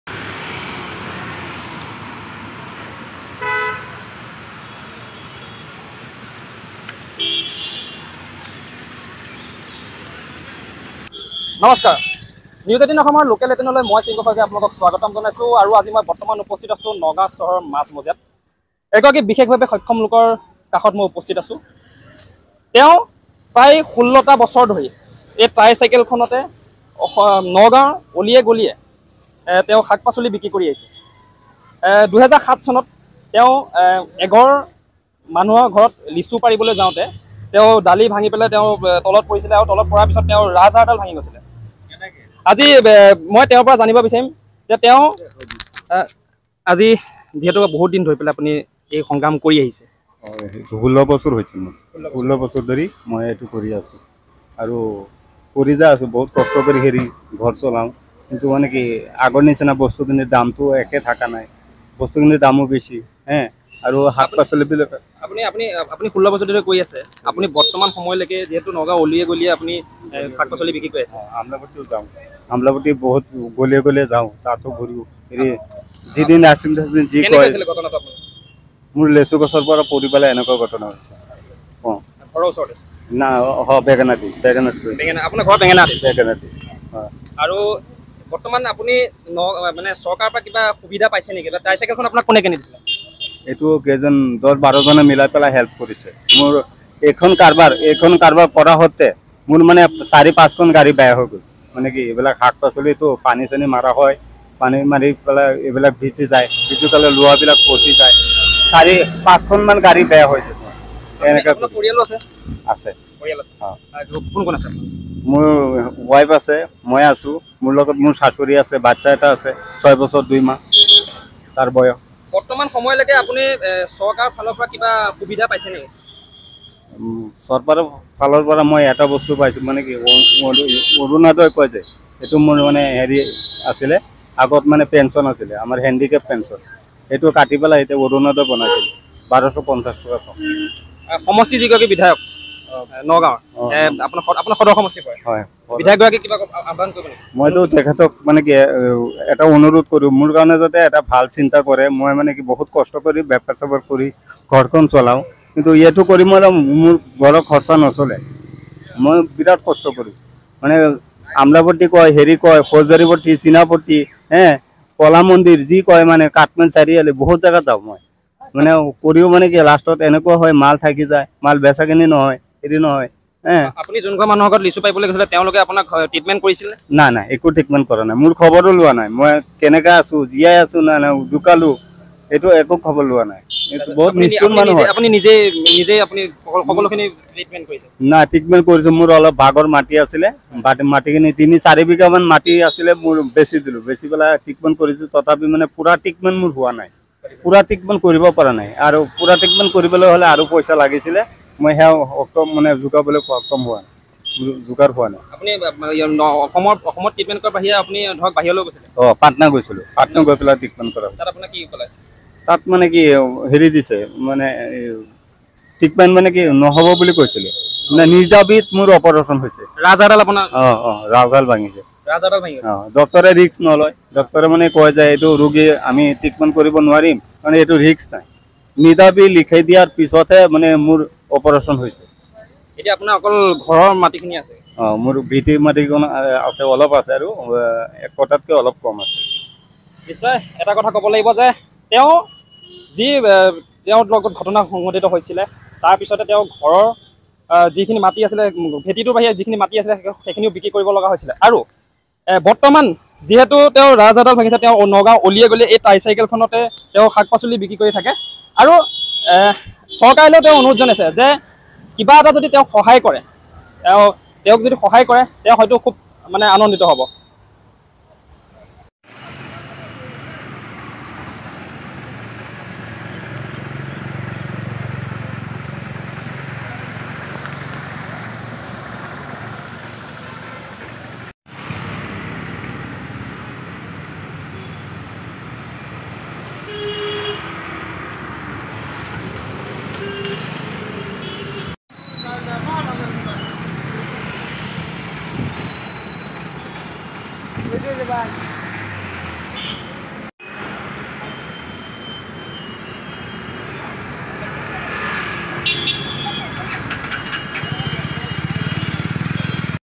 Nagaon Story| জীৱন সংগ্ৰাম! লোকজনৰ ৰাজহাড় ভঙা। তথাপিও পেটৰ তাড়নাত ৰাষ্টাত বেচিছে বিভিন্ন শাক পাচলি
Nagaon Story| জীৱন সংগ্ৰাম! লোকজনৰ ৰাজহাড় ভঙা। তথাপিও পেটৰ তাড়নাত ৰাষ্টাত বেচিছে বিভিন্ন শাক পাচলি। নগাঁৱৰ বেঙেনাআটি অঞ্চলৰ এইগৰাকী বিশেষ ভাবে সক্ষম লোকৰ জীৱন গাঁথাই সকলোৰে দৃষ্টি আকৰ্ষণ কৰিবলৈ সক্ষম হৈছে।